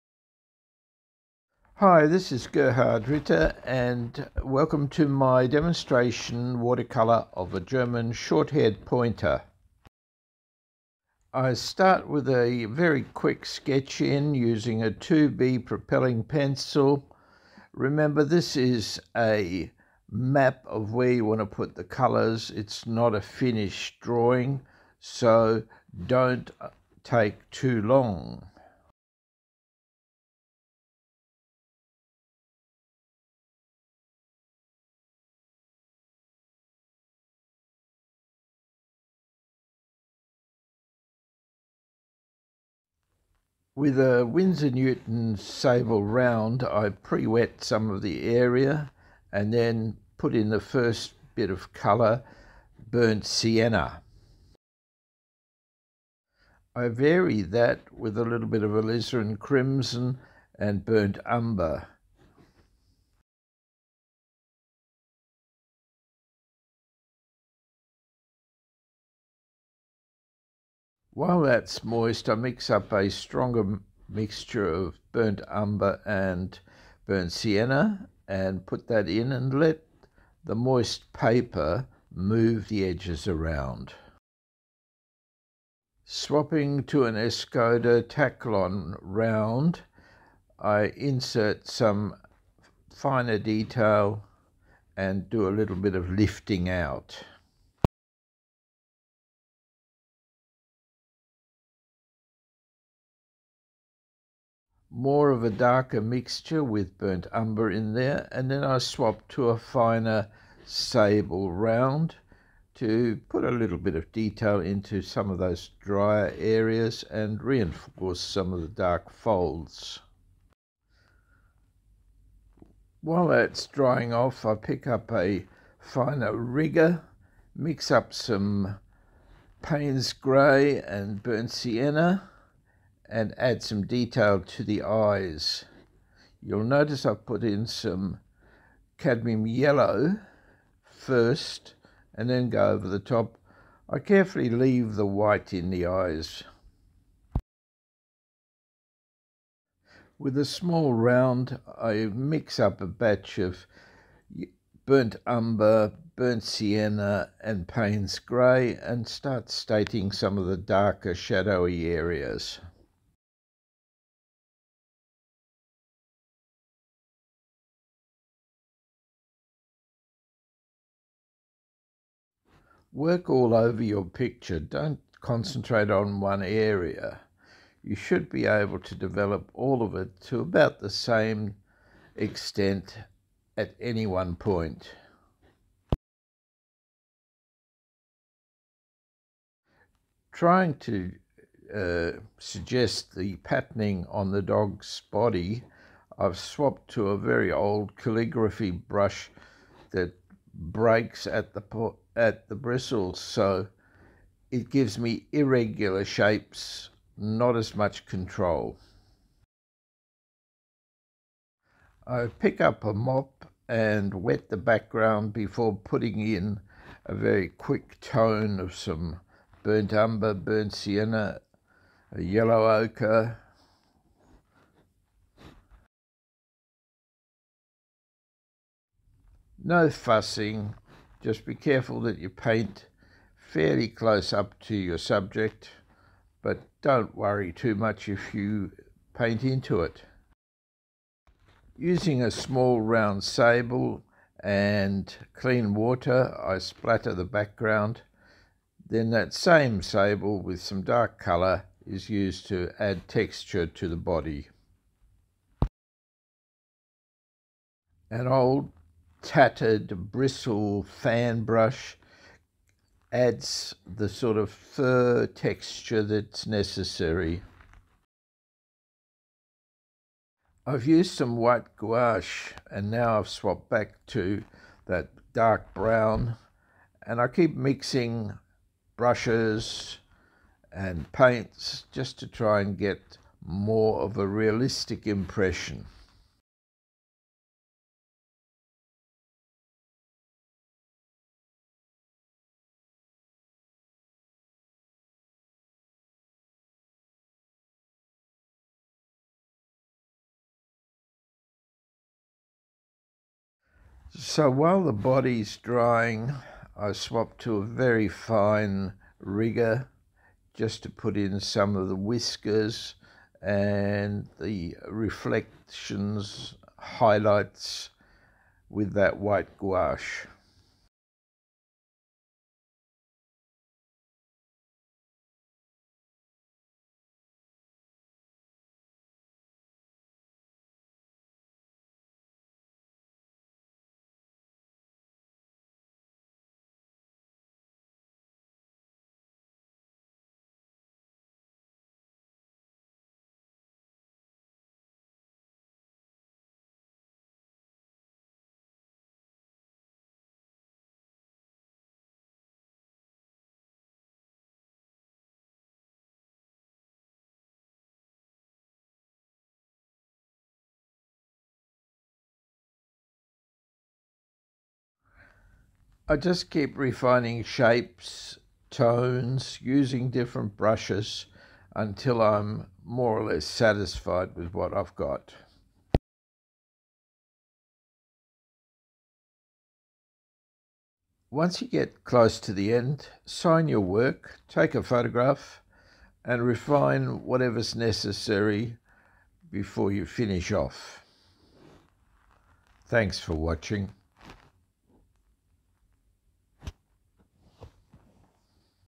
181 German Short Haired Pointer
A short demonstration watercolour painting of  a German Short Haired Pointer.